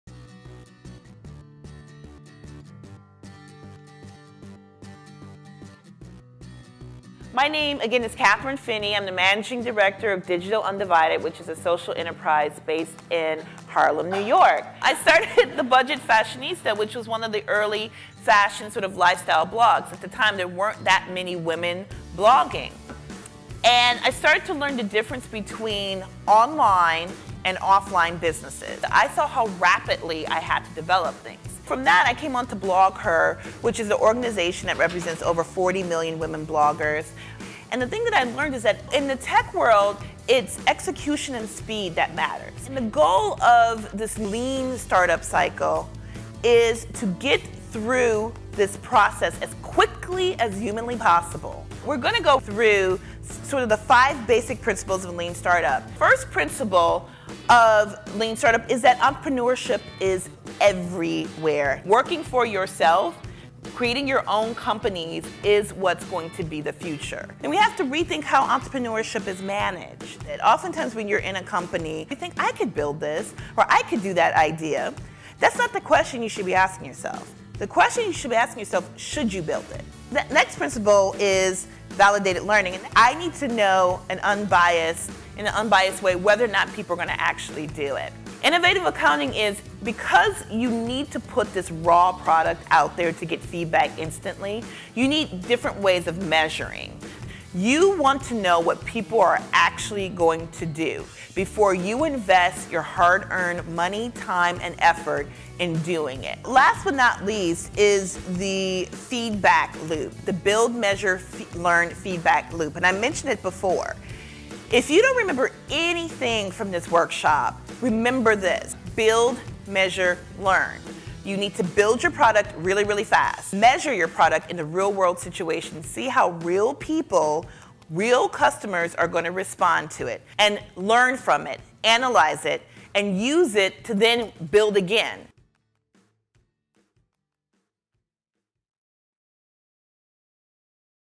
Highlights of Kathryn Finney at OPEN for Women: CEO BootCamp
Kathryn Finney of The Budget Fashionista and Digital UnDivided speaks at OPEN for Women: CEO BootCamp about how the best practices and agility of start-ups can apply to small businesses.